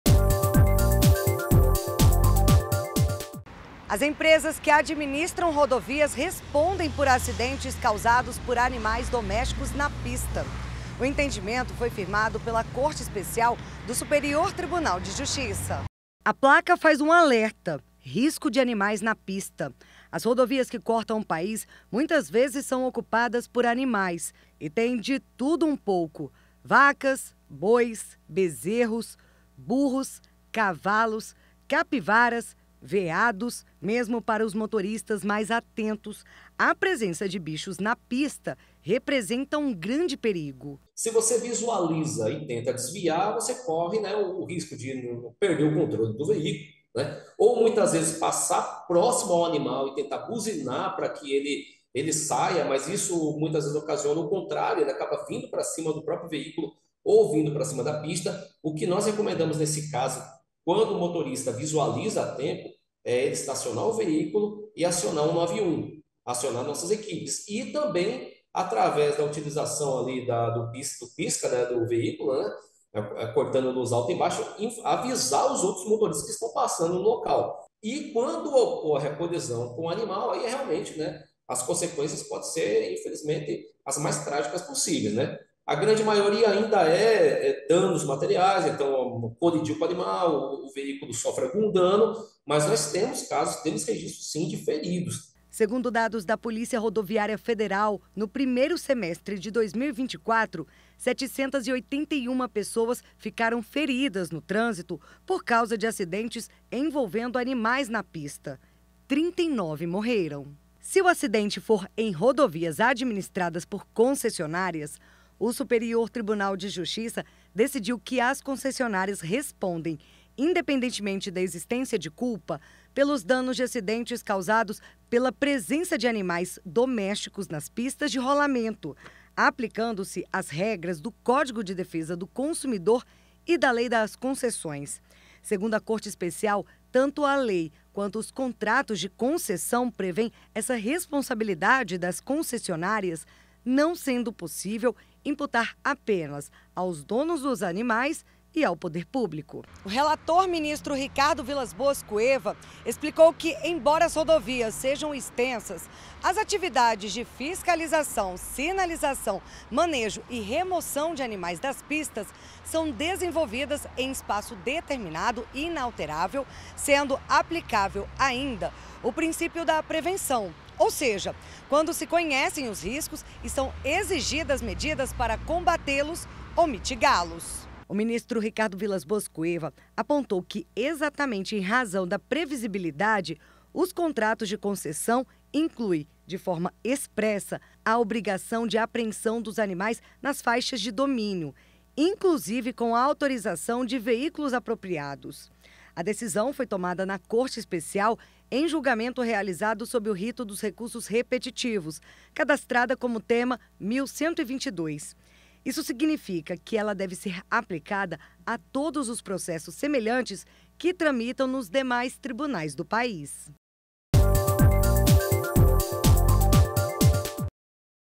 Concessionária responde por acidentes causados por animais domésticos na rodovia, decide STJ
​A Corte Especial do Superior Tribunal de Justiça (STJ), sob o rito dos recursos repetitivos (Tema 1.122), estabeleceu a tese de que as concessionárias de rodovias respondem, independentemente de culpa, pelos danos decorrentes de acidentes causados pela presença de animais domésticos nas pistas sob concessão, aplicando-se a esses casos o Código de Defesa do Consumidor (CDC) e a Lei das Concessões (Lei 8.987/1995).